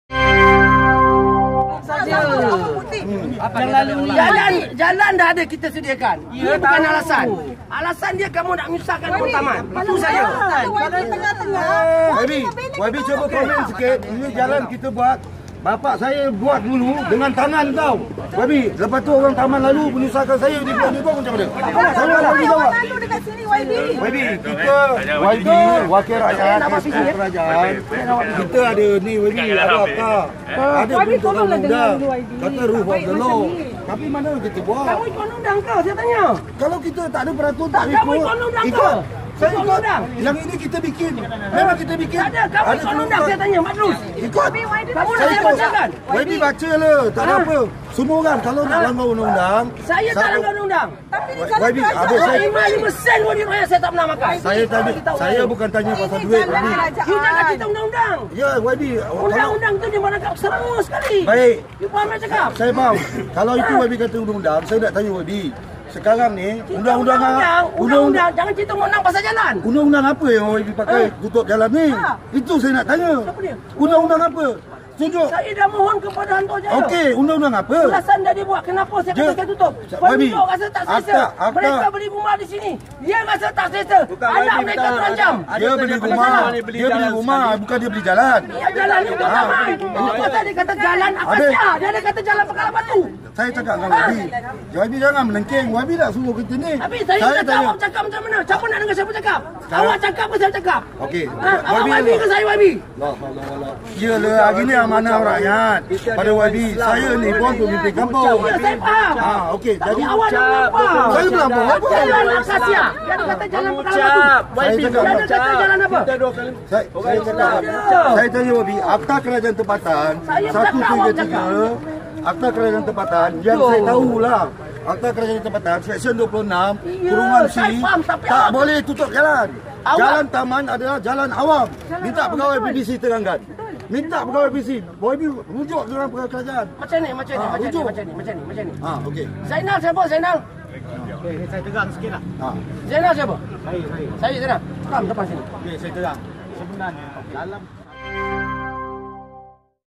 Video tular, Norhizam jelaskan kontroversi
MELAKA 9 Okt. - Pengerusi Jawatankuasa Pertanian, Pembangunan Usahawan dan Industri Asas Tani negeri, Norhizam Hassan Baktee sekali lagi menimbulkan kontroversi apabila rakaman video berdurasi 2 minit 27 saat melibatkan beliau bertikam lidah dengan sekumpulan penduduk di Taman Akasia di sini tersebar hari ini.

Kejadian berlaku pagi tadi ketika Norhizam yang merupakan Ahli Dewan Undangan Negeri (ADUN) DAP kawasan Pengkalan Batu hadir di taman tersebut bagi menghadiri sesi dialog dan sidang media berhubung penutupan jalan pintas antara Taman Akasia menuju ke Taman Kerjasama di Bukit Beruang.